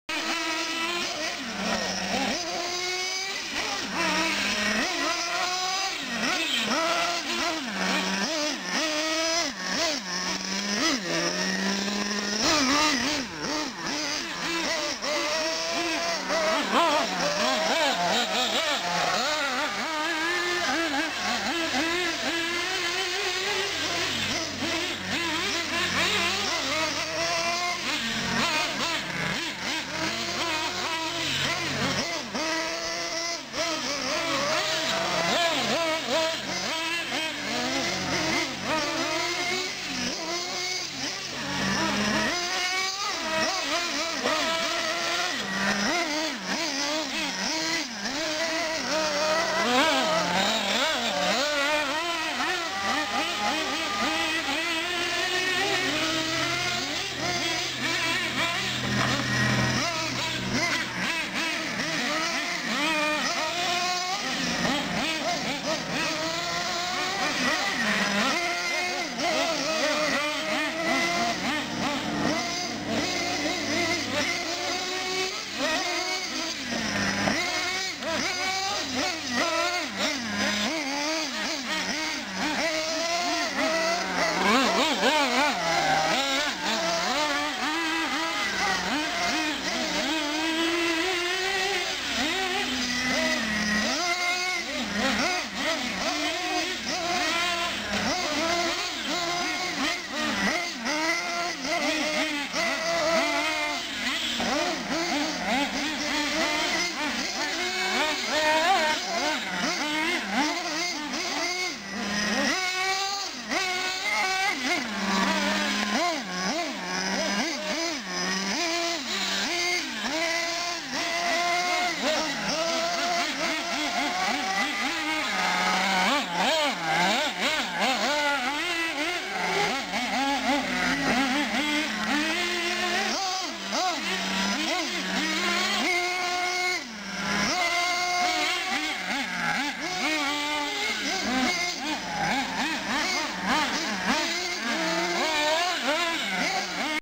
on the Outback track-- June 20, 2009 clip 1
Nitro-1/8 scale offroad SC8 style adapted trucks- Ofna conversion(Blue Body) and. TMaxx conversion(Silver Body) at the Outback track near Des Moines, Iowa June 20, 2009--Thad driving the TMaxx, Scott driving the Ofna--The TMaxx needs a little tuning